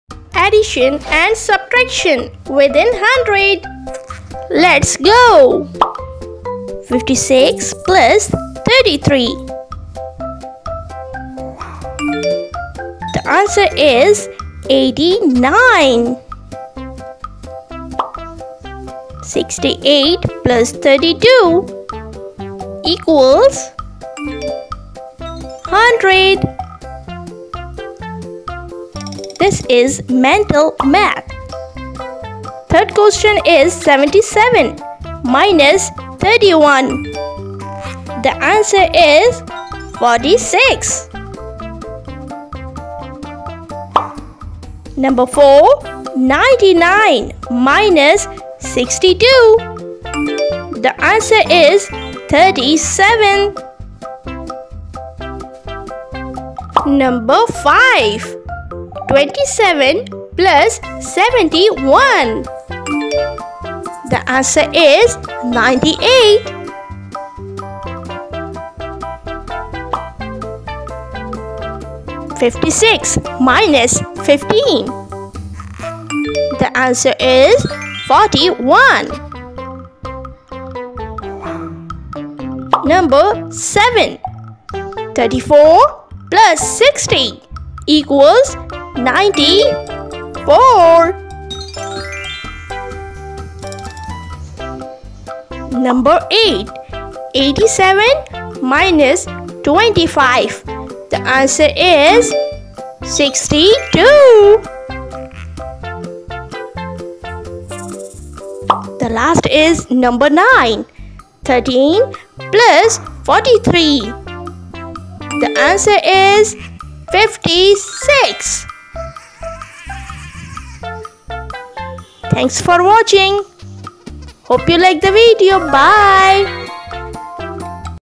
Numberblocks Double Digit Addition & Subtraction Made FUN!
Double Digit Addition & Subtraction with Number Blocks | Fun Math Practice for Kids

"Welcome to Educational Corner!
In this  video, kids can practice double-digit addition and subtraction with in 100 , with the help of colorful number blocks. It’s perfect for young learners to build confidence in their math skills while enjoying animated, engaging visuals.
channel handle: @Educationalcorner110